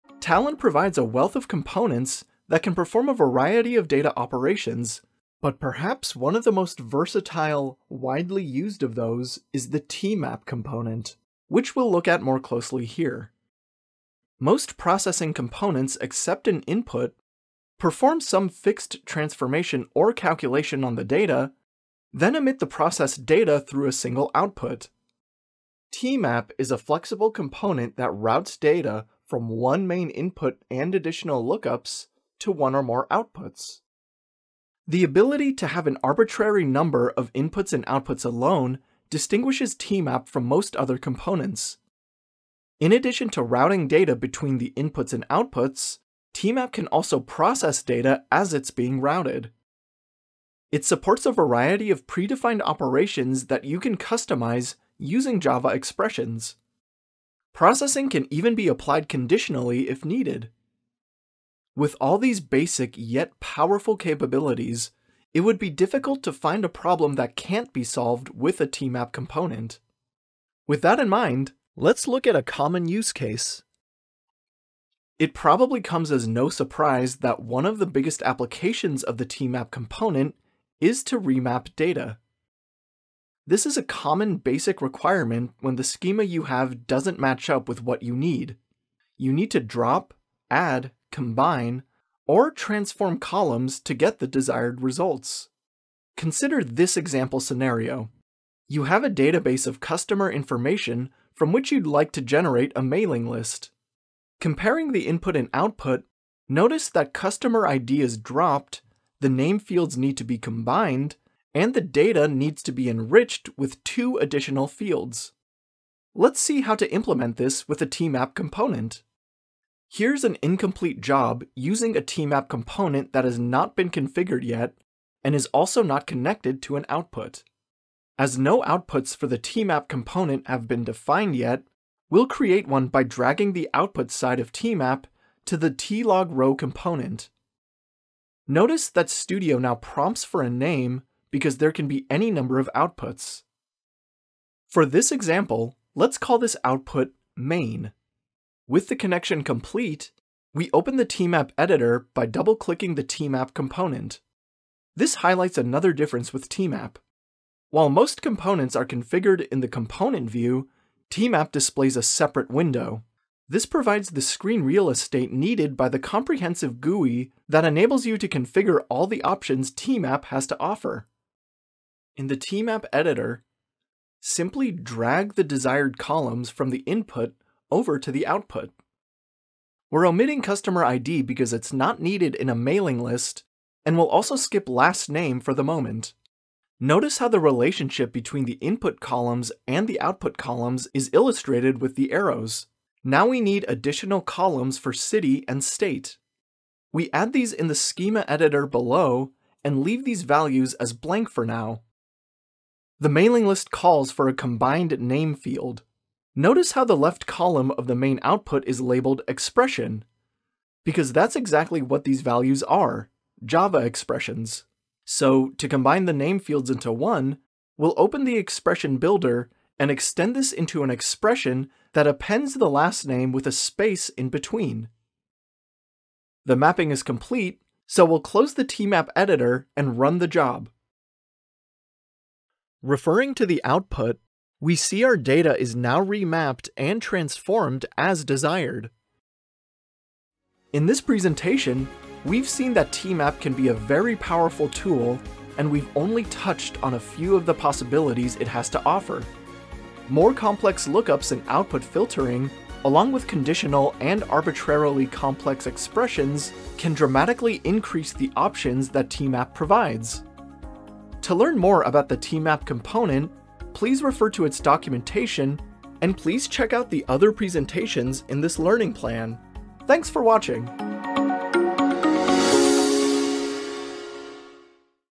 7. Introduction to tMap
tMap : Transforms and routes data from single or multiple sources to single or multiple destinations.
tMap is an advanced component, which integrates itself as plugin to Talend Studio.